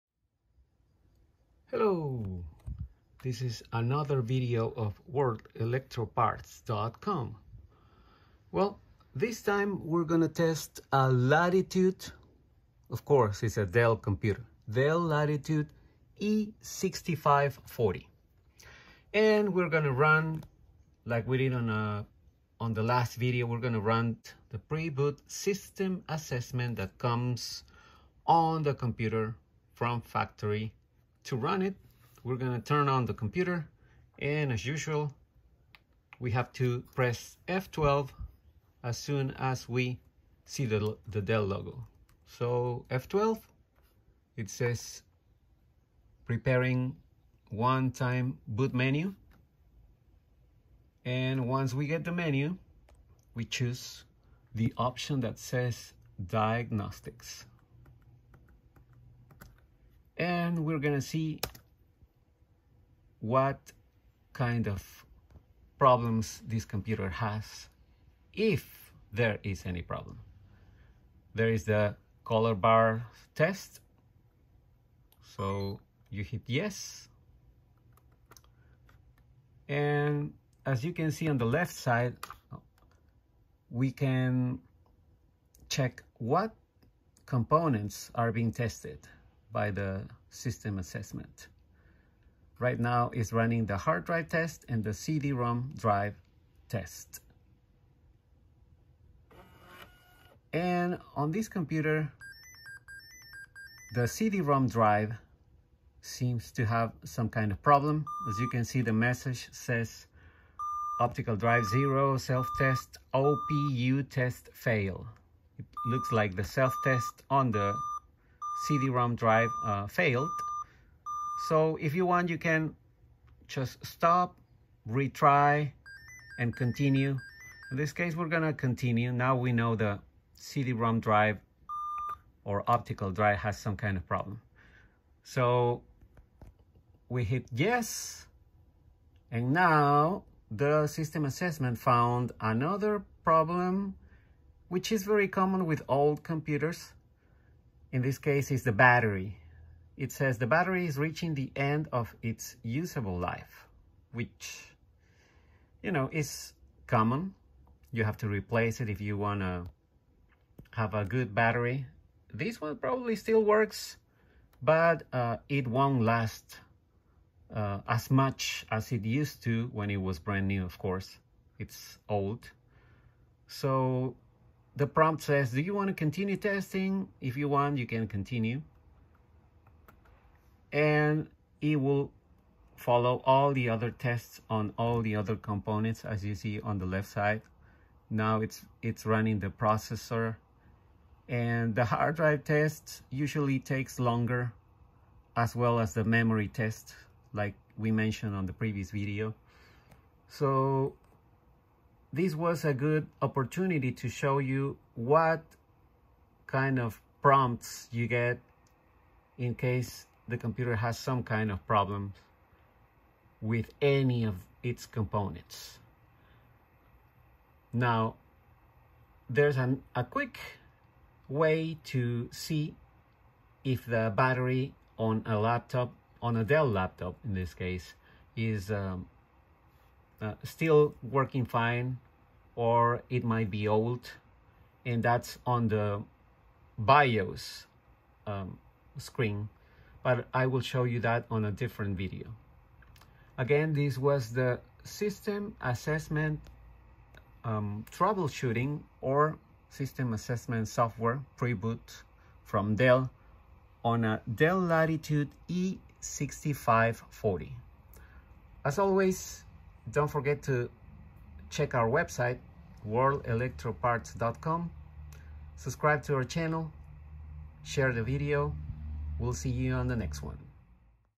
Self test on a Dell Latitude E6540 - Pre-Boot System Assessment
Watch out video to learn how to run a delft test on a Dell Latitude E6540 laptop computer